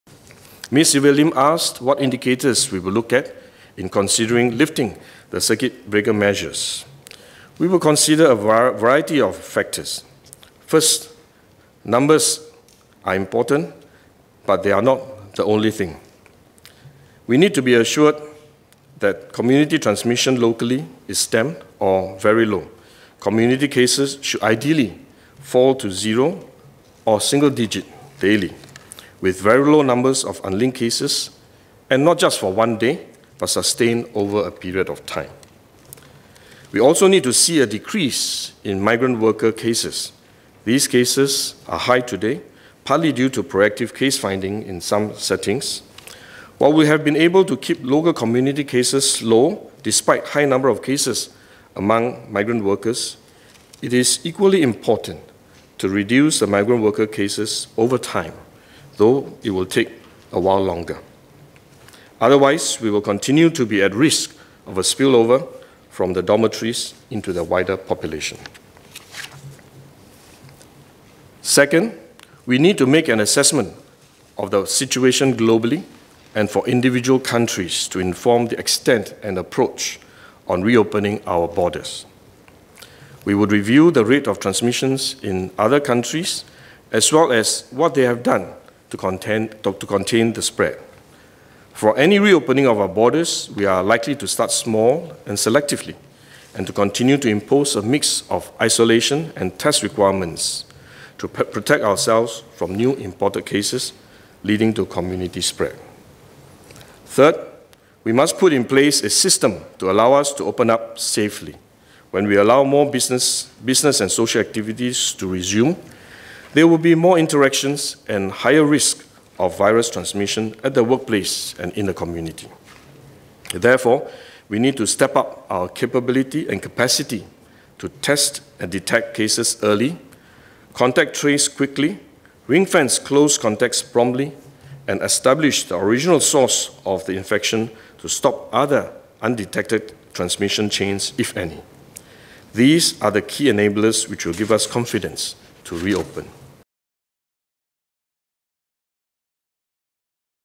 Three factors that the Government will consider before lifting circuit breaker measures
Three factors must first be considered before Circuit Breaker measures can be lifted, said Minister for Health Gan Kim Yong in Parliament on 4 May 2020.  Firstly, the daily number of locally transmitted cases must be close to zero or in the single digits, along with a decrease in migrant worker cases. Secondly, the Government will assess the global and national situation in other countries before arriving at an informed approach on reopening Singapore’s borders. Third, a system must be put in place to step up capability and capacity to contact trace quickly, in order to allow businesses and social activities to resume safely.